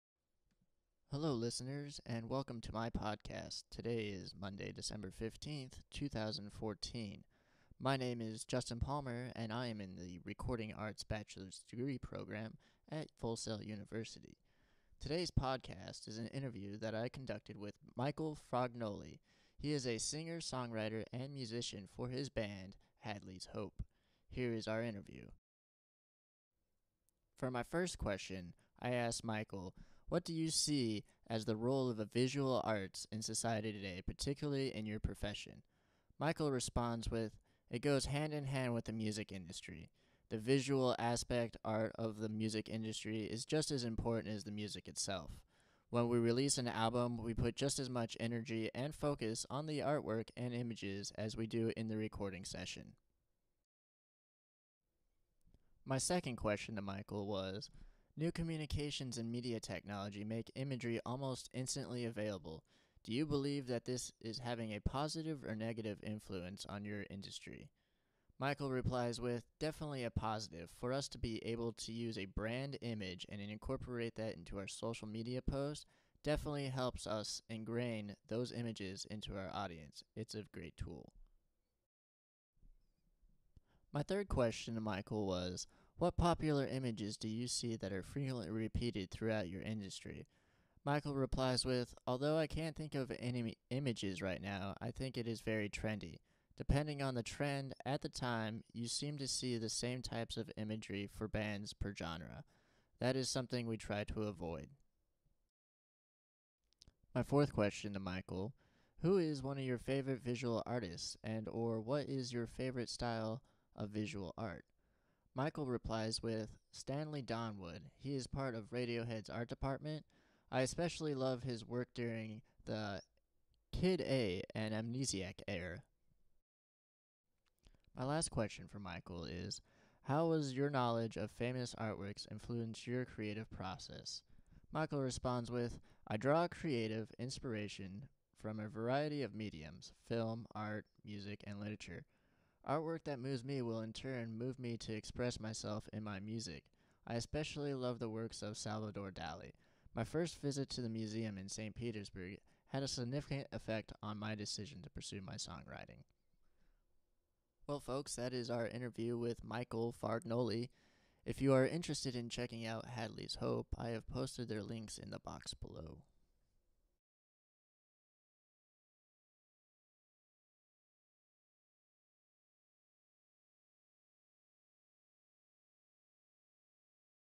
Art and the Industry Interview with Michael Fargnoli
Interview from Michael Fargnoli Singer, Songwriter, and Musician for the band Hadley's Hope